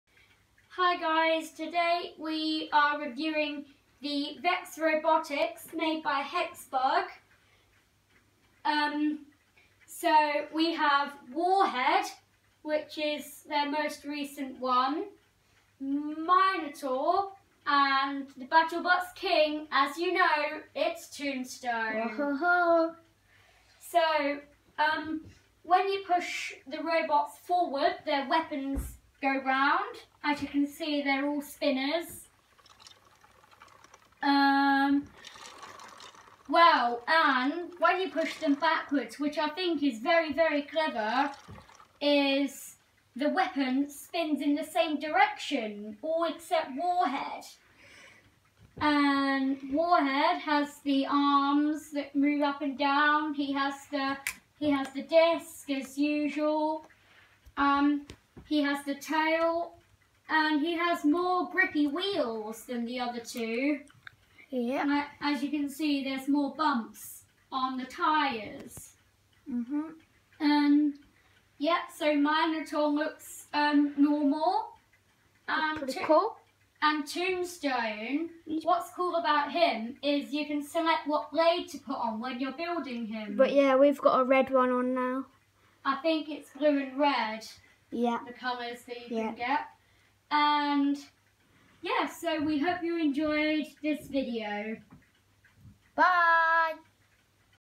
HEXBUG VEX ROBOTICS. Battlebots Warhead, Tombstone and Minotaur.
A quick look at the Hexbug Vex robotics Battlebots.
Warhead, Tombstone, Minotaur